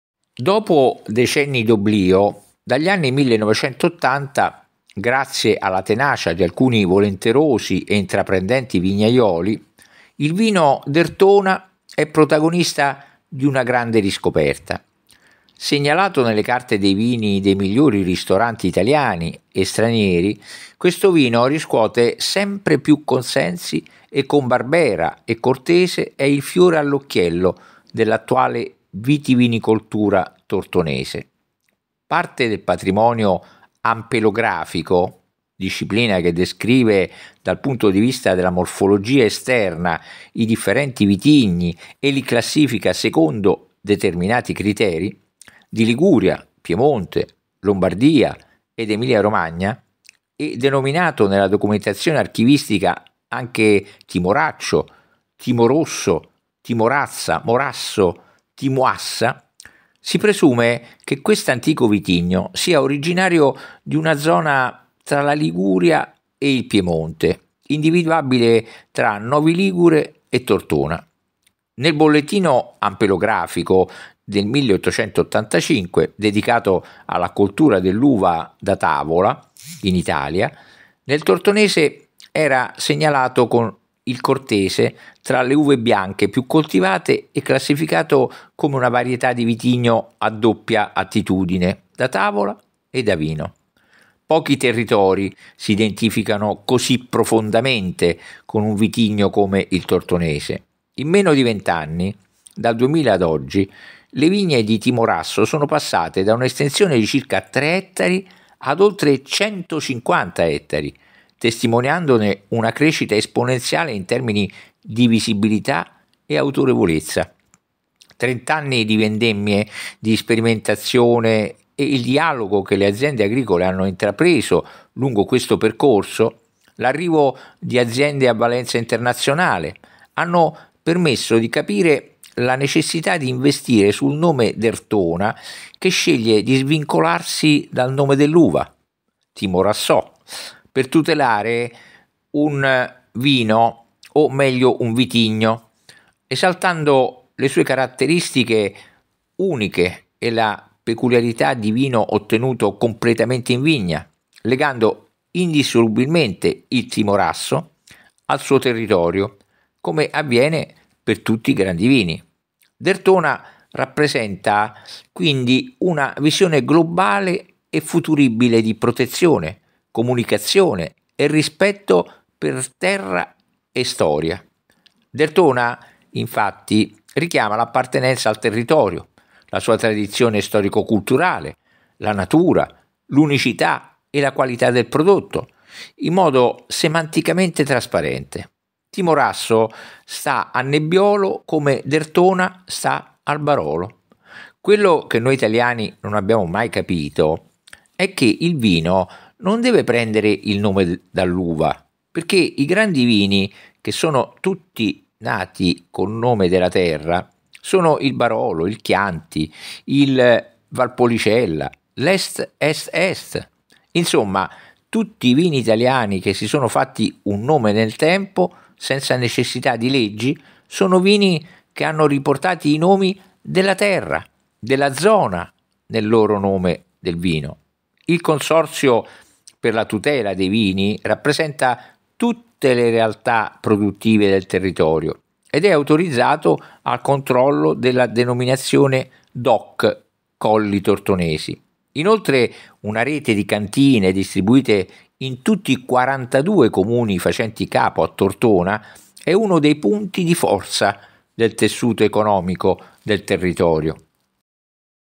Derthona: un Territorio, un Vino, un Vitigno (Piemonte) (creato con Spreaker)
Derthona, antica città romana, è il nome scelto per identificare il Timorasso dei Colli Tortonesi, riscoperto e valorizzato a partire dagli anni 1990.
Disse Veronelli: “a Garbagna bevvi vino bianco buonissimo, pieno, armonico, viperino”.
Il vitigno principe del territorio, quello che occupa la maggior superficie, è però la Barbera.